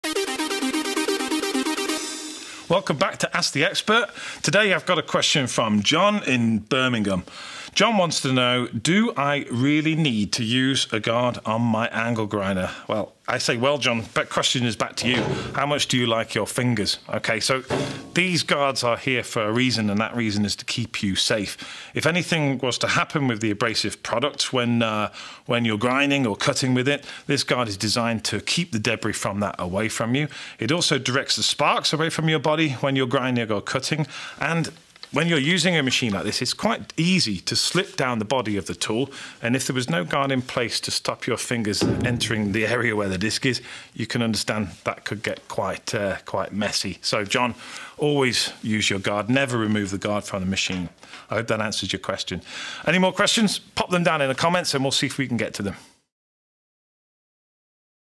ASK THE EXPERT: Do I really need to use a guard with my angle grinder?
Welcome back to 'Ask the Expert', this week, Norton Application Engineer Paul Gray is exploring the reasons why it's critical to use a guard every time you use your angle grinder.

Don't be tempted to cut corners, even if you're only doing a short cutting job or a bit of grinding. The guard should never be removed from your angle grinder. It's designed to protect you! It keeps cutting and grinding debris away from you and directs sparks away from your body. It's also easy for your hands to slip down the grinder, especially when you're applying lots of pressure, and the guard will stop your fingers entering the area where the disc is rotating.

Always check the latest FEPA safety regulations if you're unsure of what equipment to use.

If you'd like to 'ask the expert' and put Paul to the test, drop your question in the comments box below and we'll get back to you with an answer!

or contact your local Norton distributor.